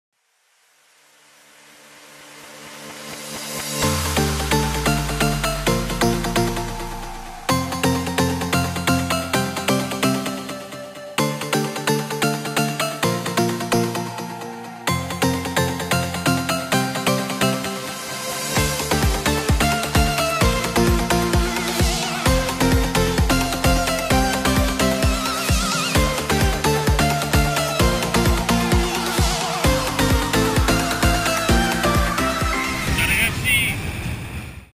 Ciclomaretona 2018 - Viareggio-Senigallia - Highlights
Un breve condensato del viaggio on the road da Viareggio a Senigallia, primo capitolo della saga "Ciclomaretona"... Let's ride with us!